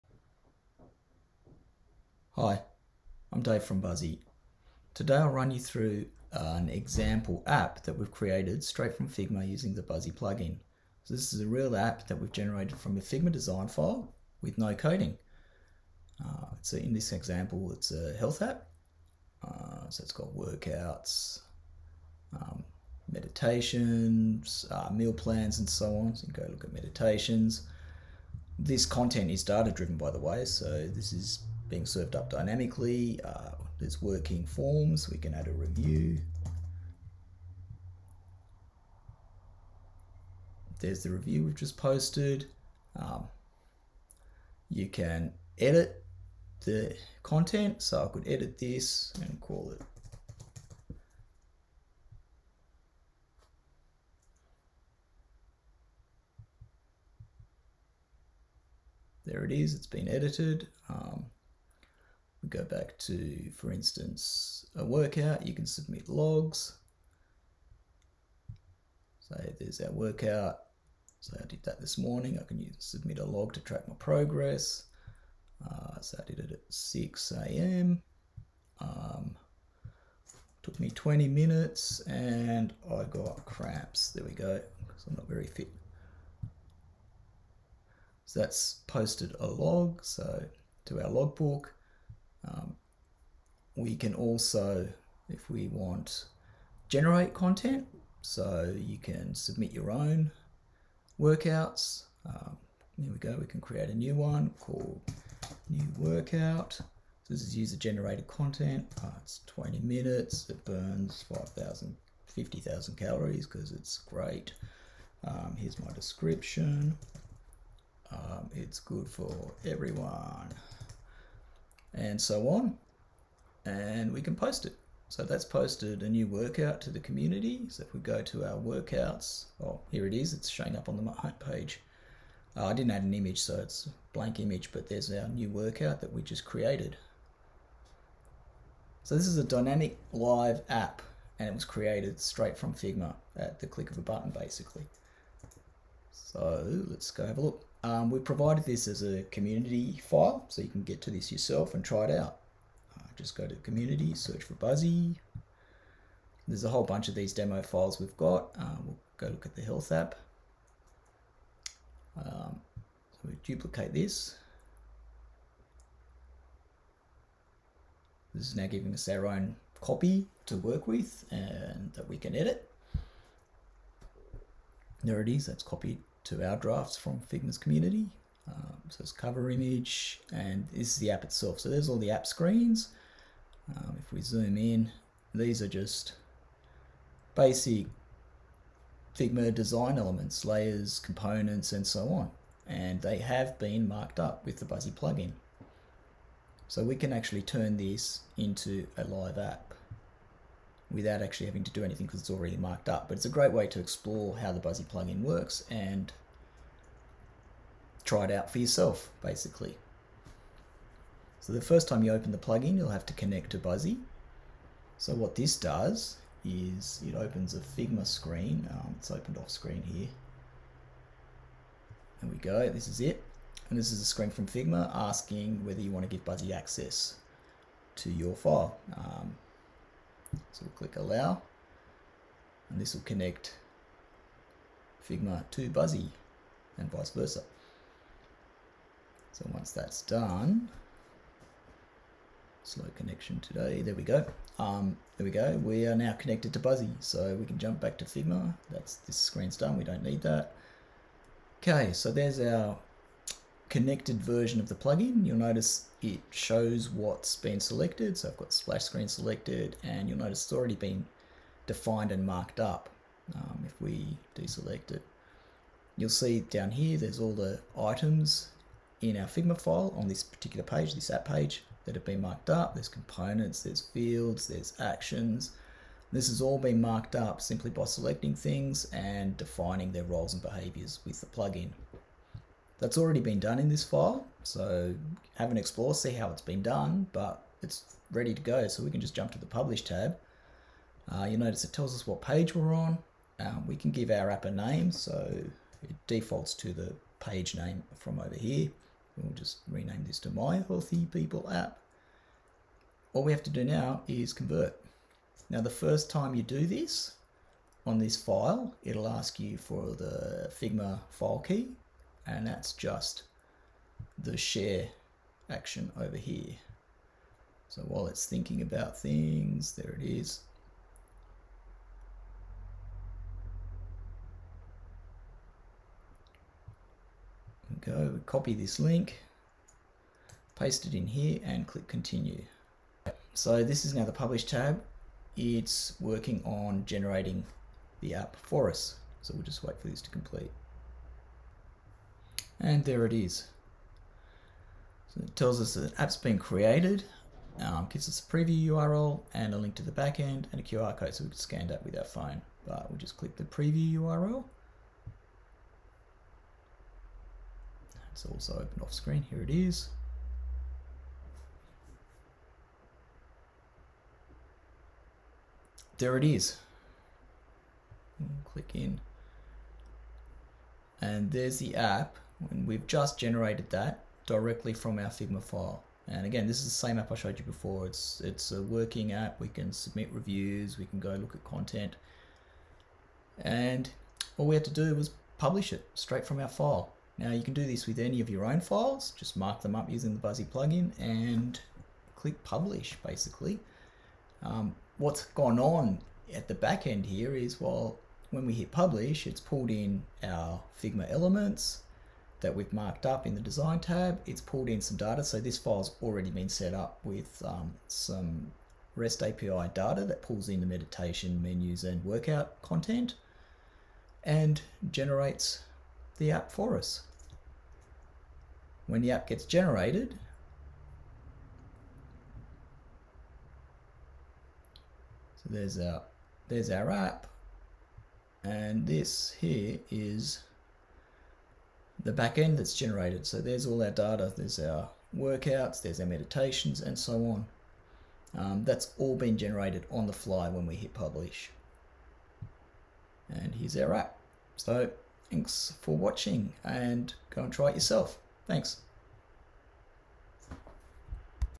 Buzzy Figma Plugin: Example health app walk-through
A walk-through of a live health app example created straight from a Figma design. This file is a demo file we provide in the Figma community, already marked up with the Buzzy plugin. Simply grab the Figma file, install and connect the Buzzy plugin, and hit Publish to create your own version - this generates a live app for you, including working forms, and dynamic content pulled from Rest APIs.